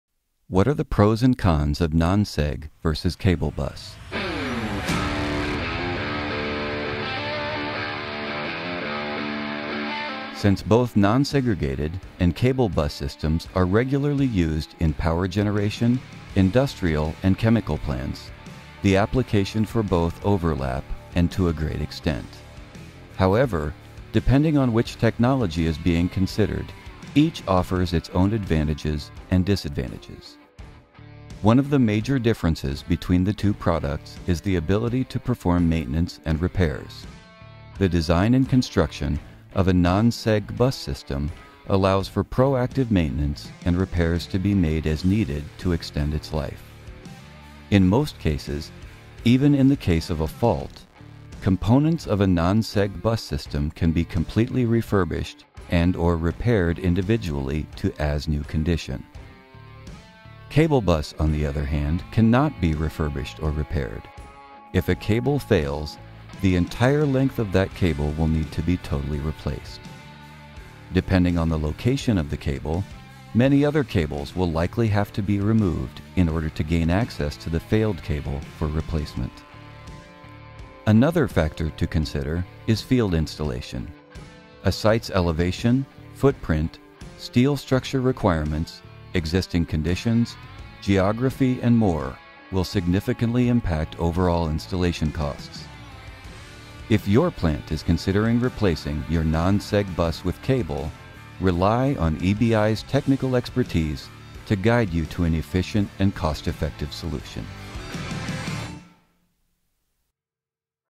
Cable Bus vs. Non-Segregated Phase Bus
Is your plant is considering replacing your existing non-segregated bus system with cable bus technology? If so, you’ll want to tune into our 6th installment in our Q&A Vlog Series as we discuss the pros and cons of non-seg and cable bus duct systems. Want more information on this topic? Take advantage of scheduling a webinar with members of our technical team. Our technical presentation entitled: “Cable Bus vs. Non-Segregated Phase Bus”, is just one of many free webinar topics available.

To view all the Q&A videos in this series, visit us